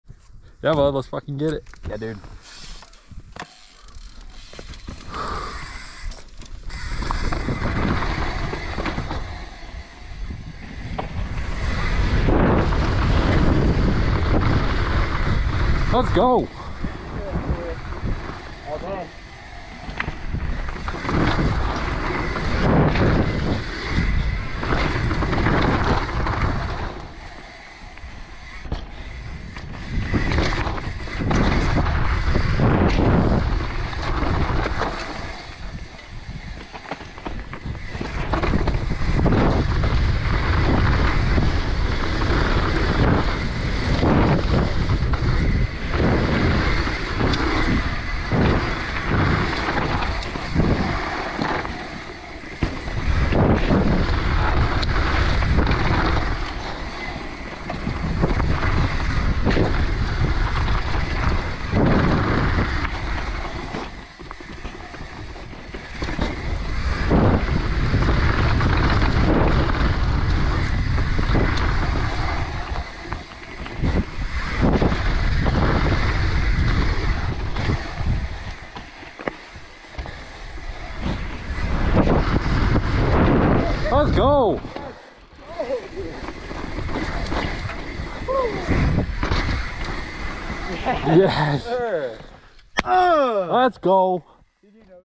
THE CRAZIEST START TO A RUN // Downhill Mtb
This is the craziest start to a run! Dropping off the ‘incinerator’ (a drop that took us over 9 days to build) right into the rest of the ridge from the original Red Bull Rampage!

If you like this video check out a few similar ones on my page!